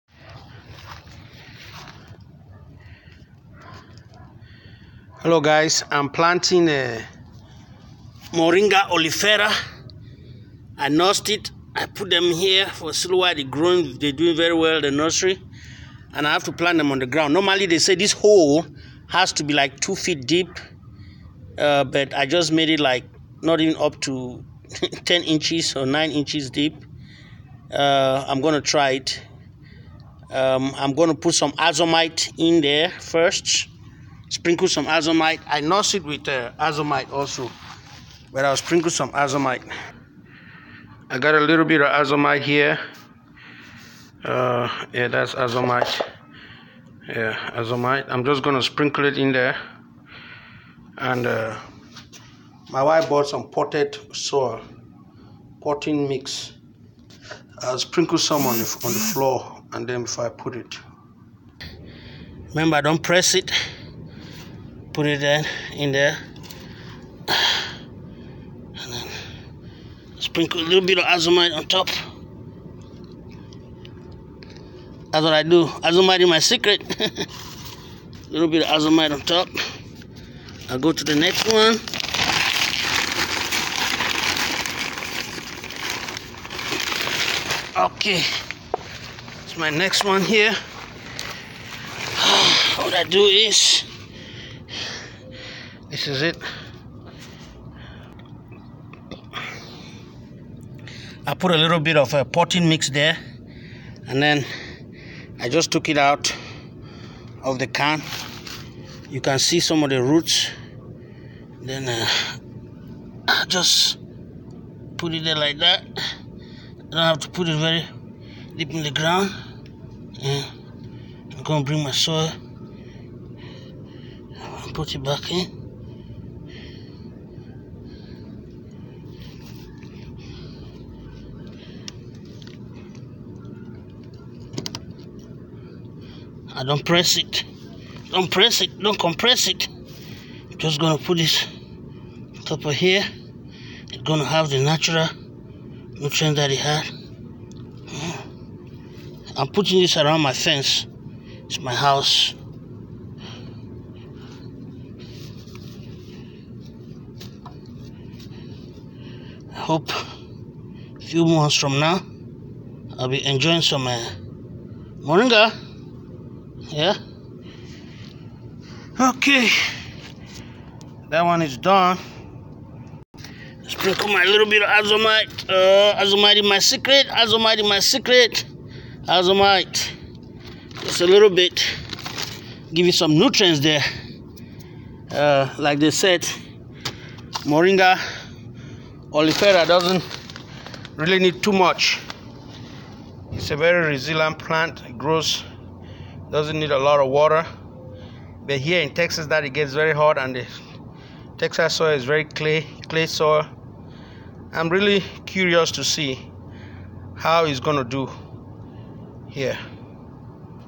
Planting Moringa Olifera with AZOMITE
Planting Moringa Olifera in Dallas Texas USA with AZOMITE June 6, 2019.